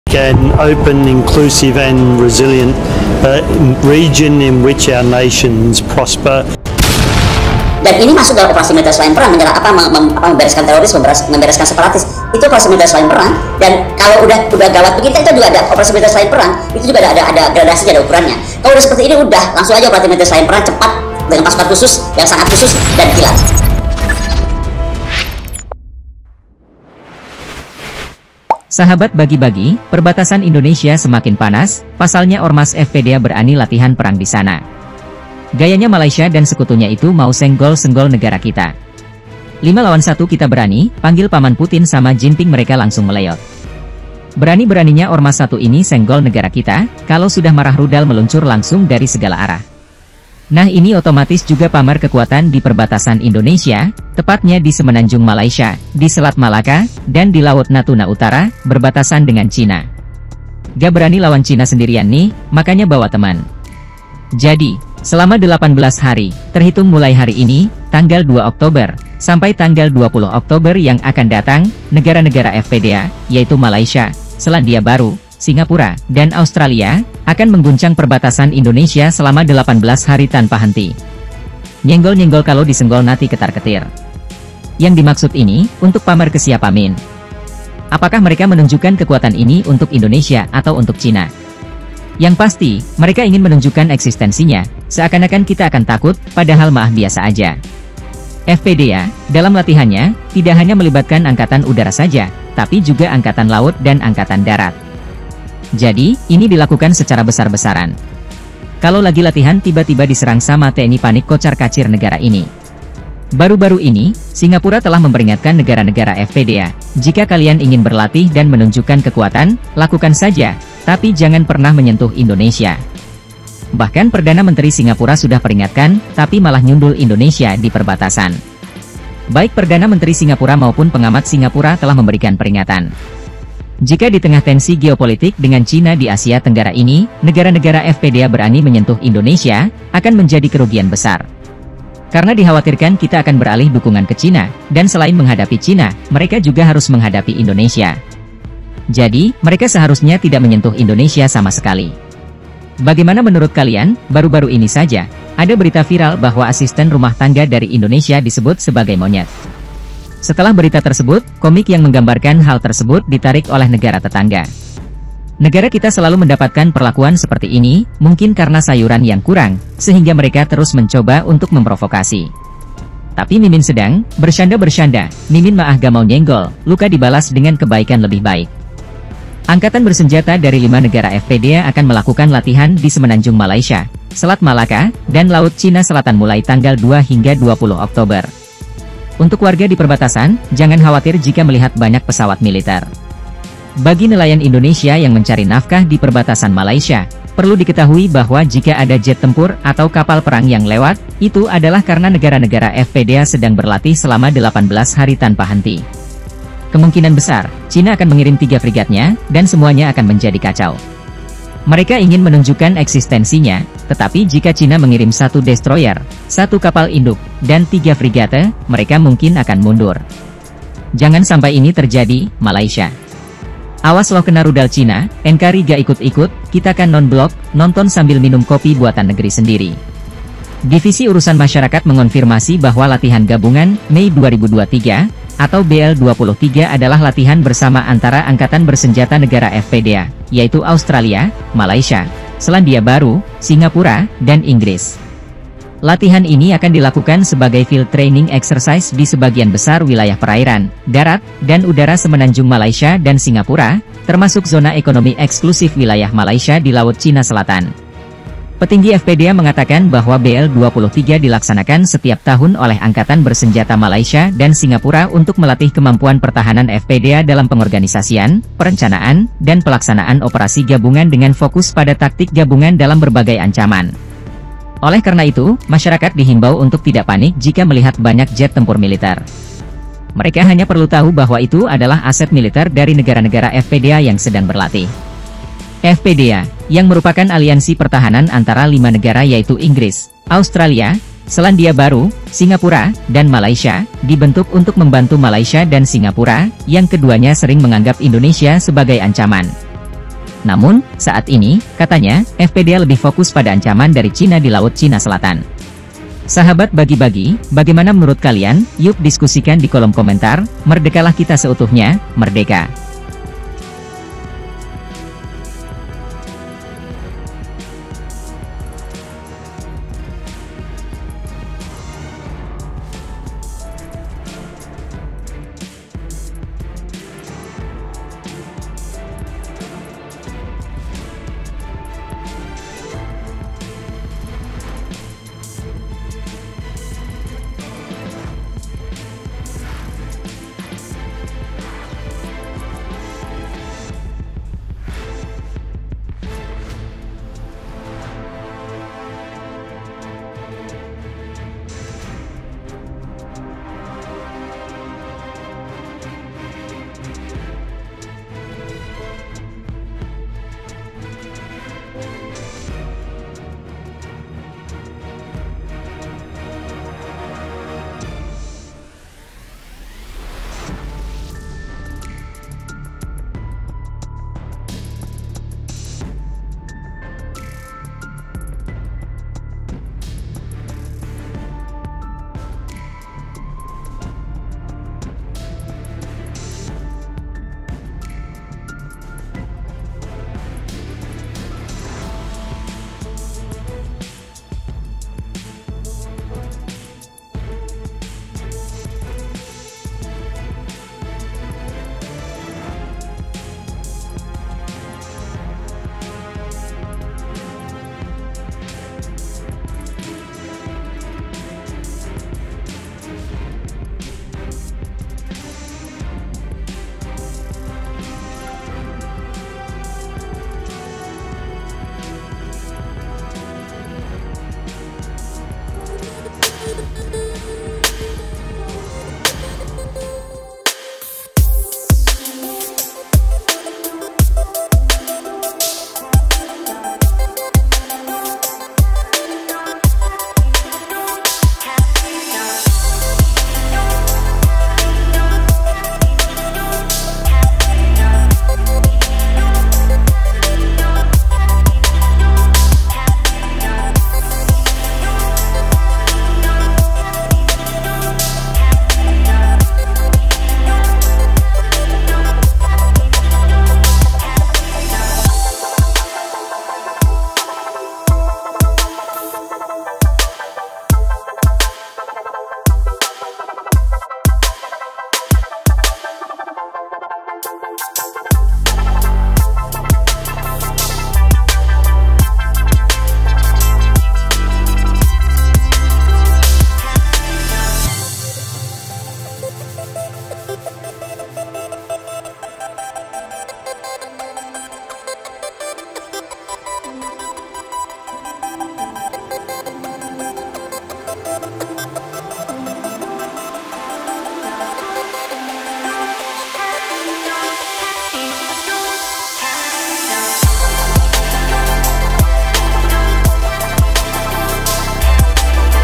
AUTRALIA, SINGAPUR DAN MALAY KEMBALI RUSUH ANCAM NKRI! FPDA LATIH SKIL PERANG DI PERBATASAN RI!
Catatan :
"Kemerdekaan hanyalah didapat dan dimiliki oleh bangsa yang jiwanya berkobar-kobar dengan tekad ‘Merdeka, merdeka atau mati !" - Bung Karno
PENTING!!